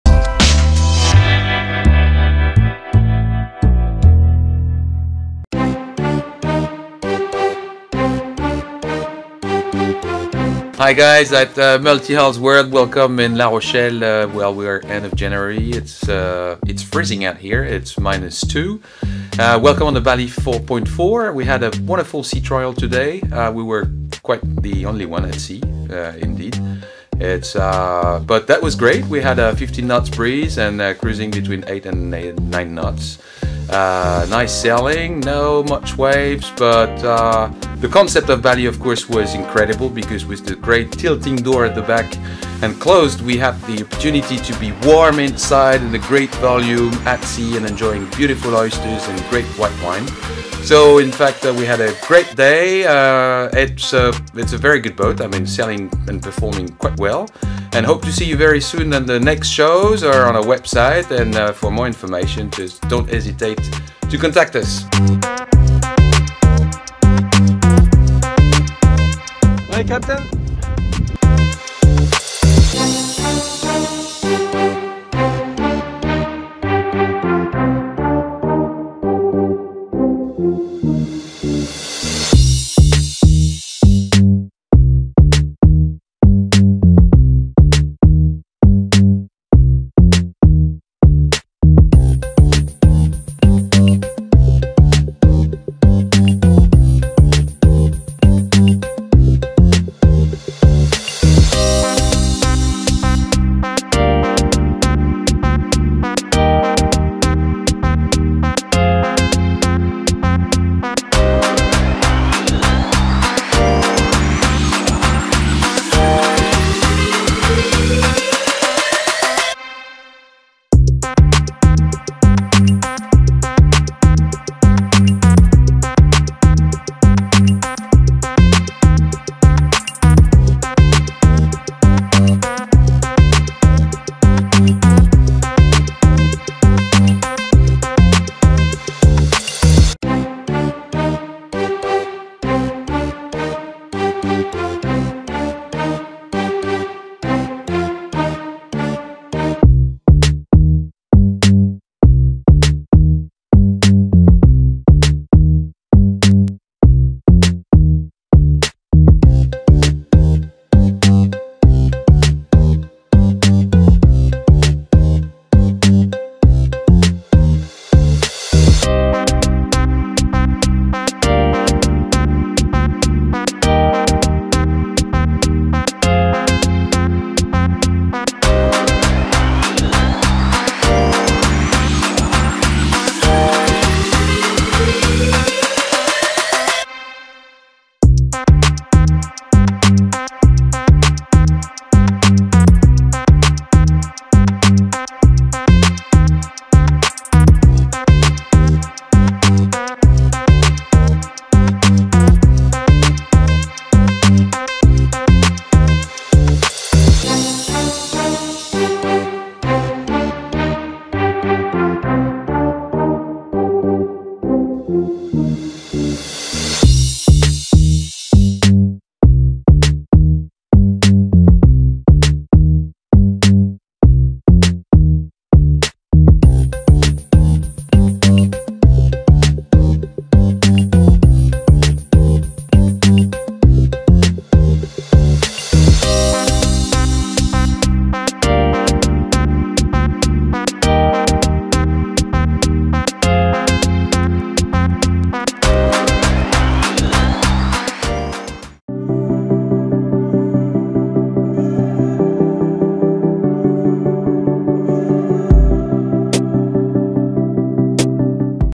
BALI 4.4 Catamaran - Exclusive boat review - Multihulls World
► You can discover the very first Bali 4.4 sea trials in La Rochelle, France.
The Bali 4.4 replace the older 4.5. In the current range, it sits between the 4.2 and the 4.6. This new model will feature all the innovations specific to the brand: rigid foredeck, opening windows, tilting saloon/cockpit door and fixed aft platform to optimize the available deck area of 946 square feet (87.9 m²). The 4.4 also adopts a forward door and a flybridge with a significantly lowered helm station. Inside, 3 or 4 cabins, a large L-shaped galley and a saloon that can accommodate ten people.

► TECHNICAL SPECIFICATIONS
Builder: Bali Catamarans
Overall length: 45.1’ (13.75 m)
Waterline length: 42.6’ (12.97 m)
Beam: 24.3’ (7.40 m)
Draft: 3.9’ (1.20 m)
Light displacement: 28,200 lbs (12.8 t)
Mainsail: 818 sq ft (76 m²)
Code 0: 797 sq ft (74 m²)
Solent: 452 sq ft (42 m²)
Fuel: 211 US Gal (800 l)
Water: 211 US Gal (800 l)
Engines:  2 x 40 or 57 HP
CE Category: Cat A (Pending)